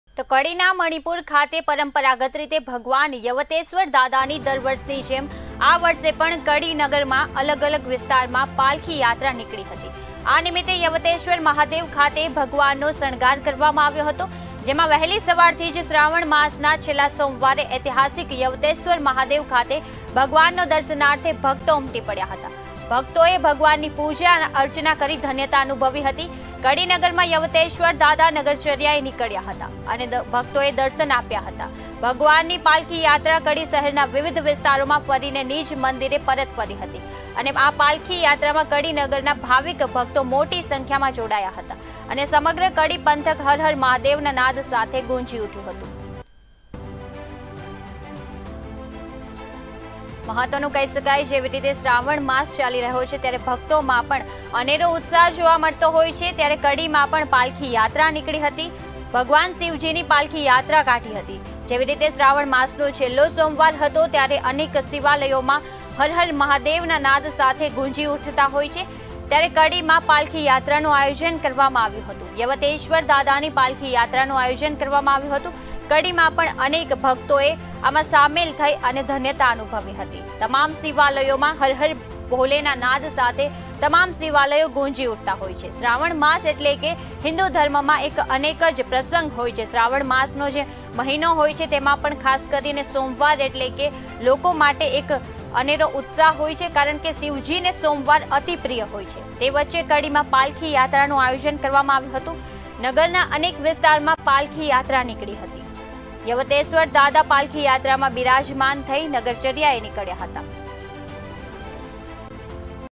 GANDHINAGAR : કડી નગરમાં અલગ અલગ વિસ્તારમાં પાલખી યાત્રા નીકળી
કડીના મણિપુર ખાતે પરંપરાગત રીતે ભગવાન યવતેશ્વર દાદાની દર વર્ષની જેમ આ વર્ષે પણ કડી નગરમાં અલગ અલગ વિસ્તારમાં પાલખી યાત્રા નીકળી હતી. આ નિમિતે યવતેશ્વર મહાદેવ ખાતે ભગવાનનો શણગાર કરવામાં આવ્યો હતો.